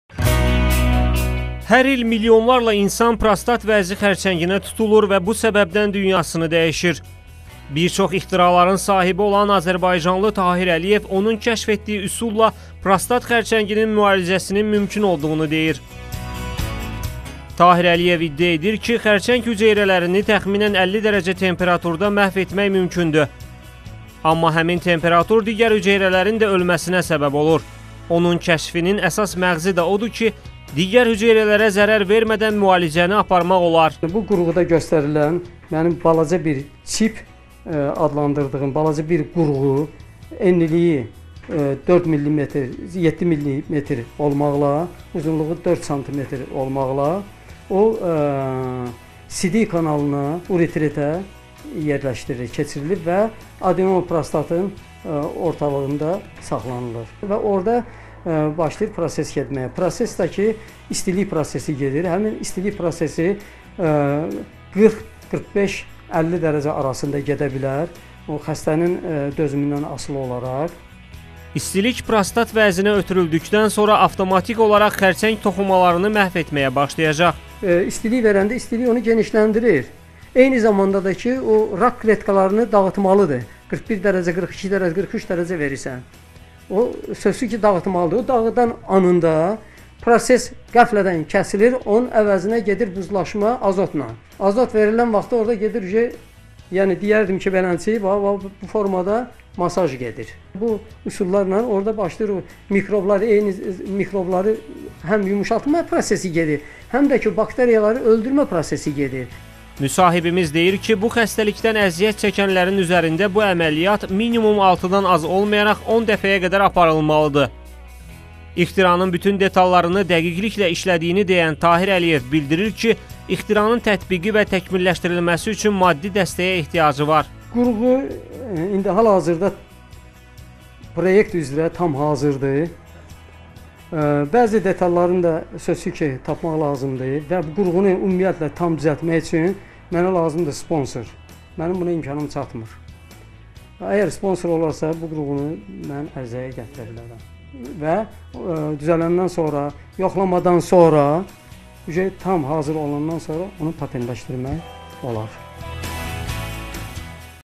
xerceng xesteliyinin caresi tapıldı
İxtiracı Tahir Eliyevin bir nece ixtirası var. O, bu ixtira ile xerceng xestelerini müalice edeceyini deyir